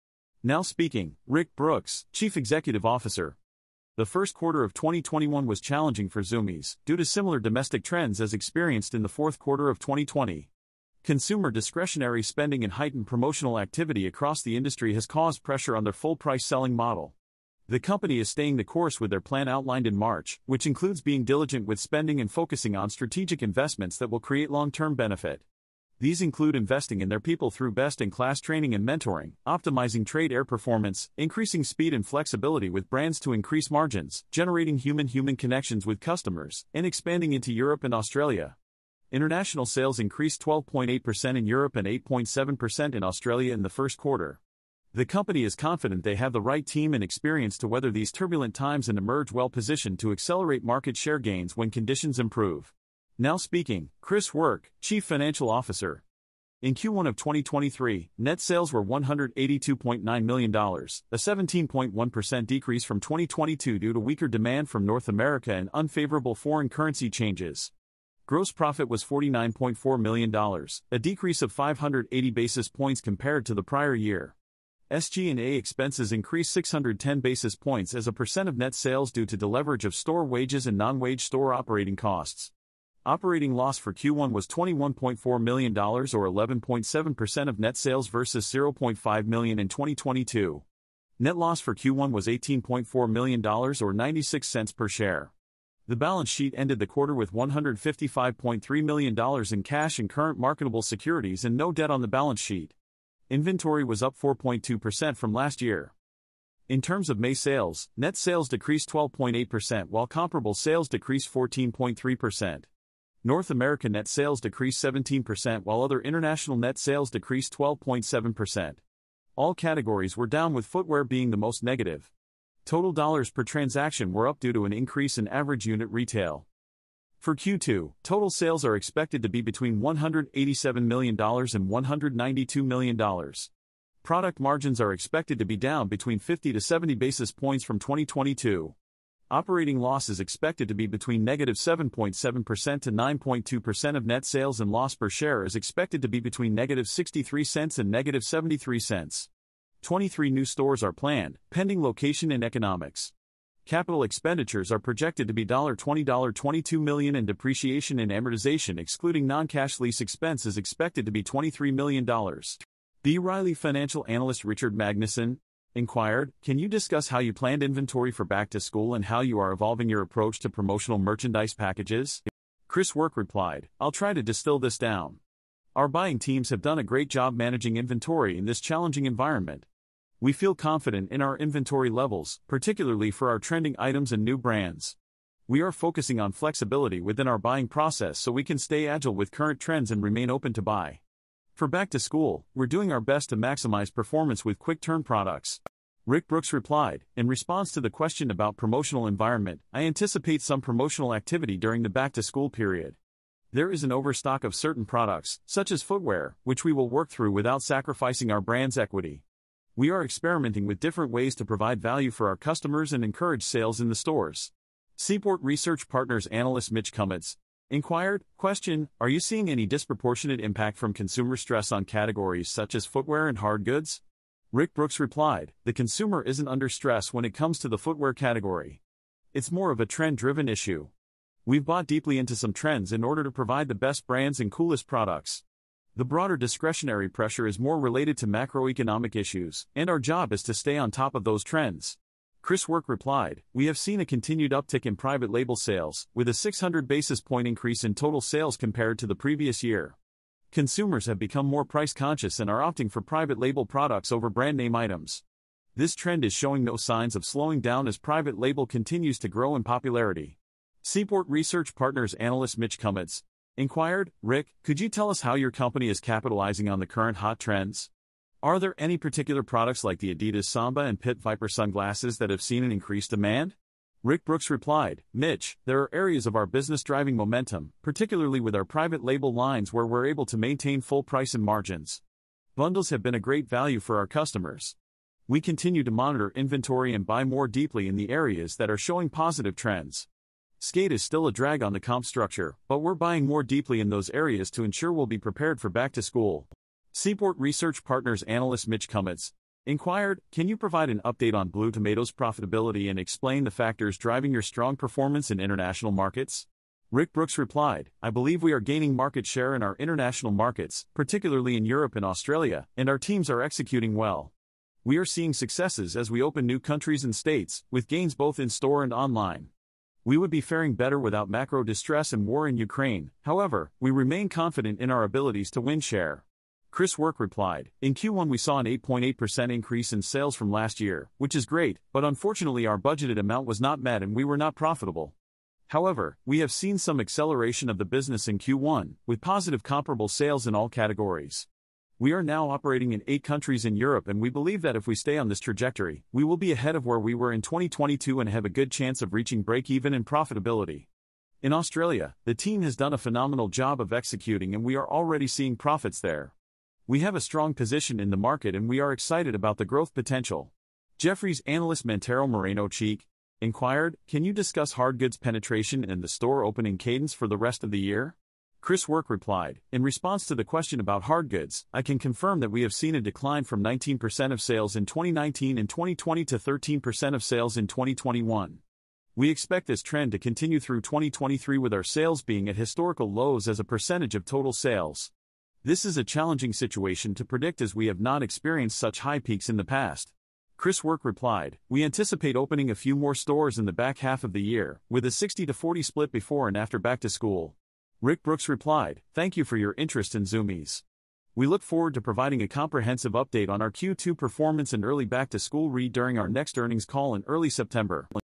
Zumiez (ZUMZ) Q1 2023 Earnings Call Summary
This is an abridged, summarized, narrated version of the Zumiez (ZUMZ) Q1 2023 Earnings Call - run time 7 minutes